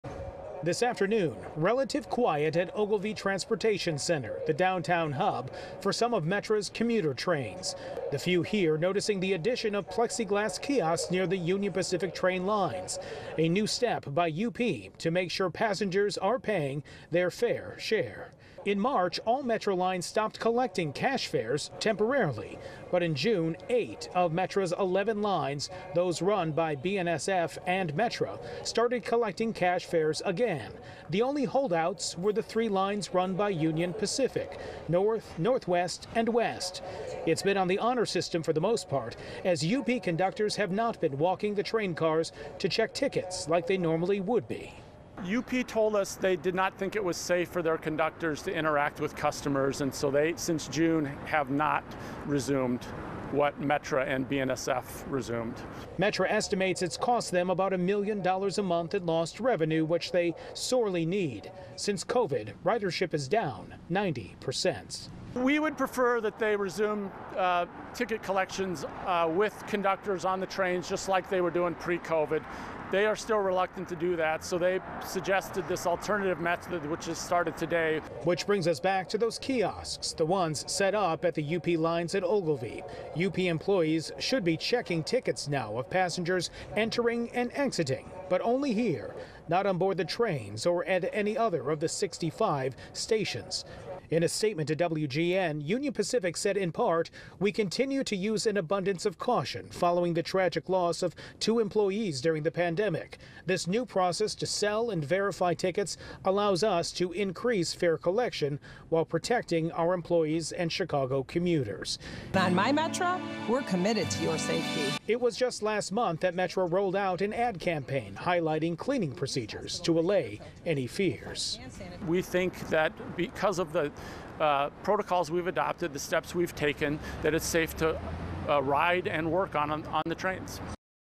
Metra's Union Pacific line to resume fare collection at Ogilvie for first time since March
With Metra ridership down more than 90 percent since the beginning of COVID-19 related lockdowns, financial struggles have hit the commuter rail system particularly hard.